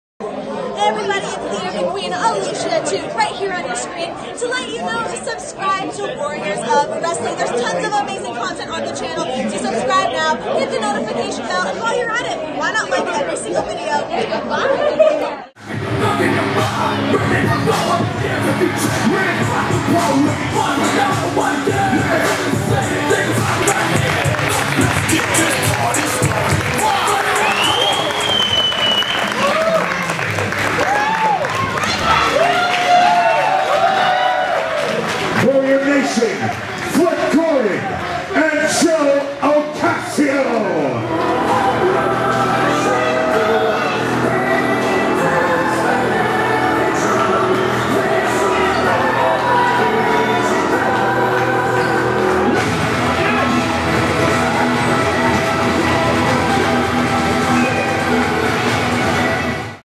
Joe Ocasio & Flip Gordon post match
Post match show of respect between Joe Ocasio & Flip Gordon after their match with W.O.W World Heavy Weight champion "The All Father" Darius Carter at  Warriors of Wrestling presents "Hostile Takeover" back on 4/9/22...